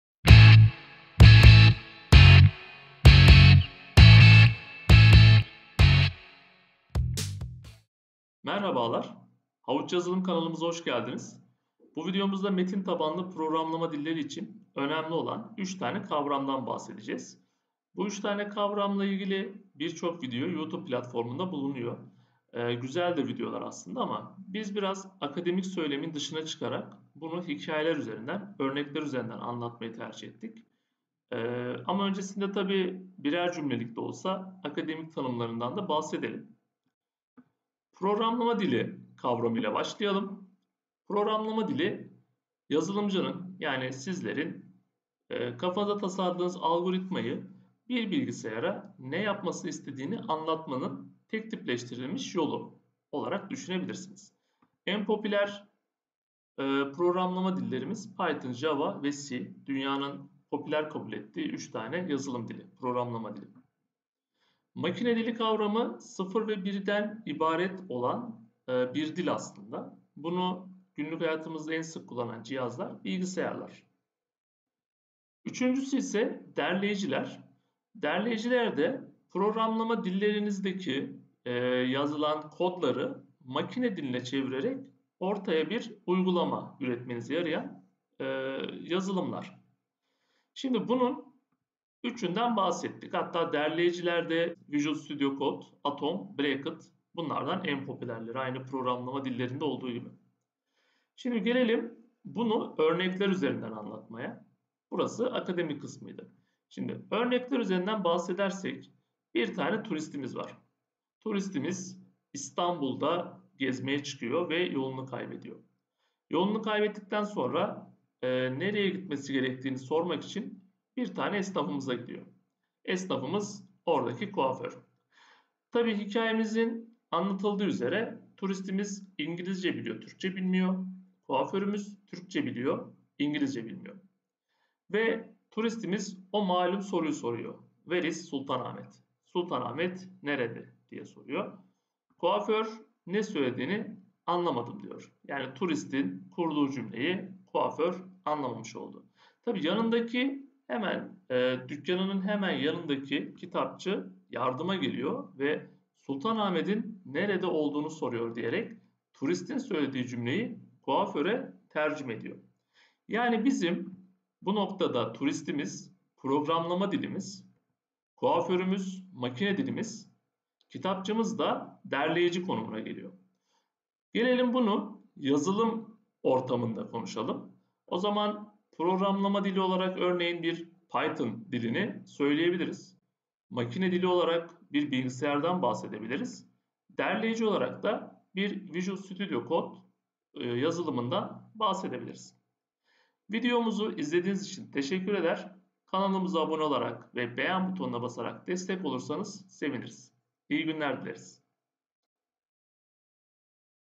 Programlama Dili, Derleyiciler ve Makine Dili Nedir? (Türkçe alt yazı desteği vardır.)
Videomuzun içeriği;
Metin tabanlı kodlamaya başlamadan önce programlamaya dair bilmemiz gereken bazı kavramlar bulunmaktadır. Bu kavramların yer aldığı, eğlenceli metaforlar ile kolaylıkla anlaşılabilecek bir video hazırladık.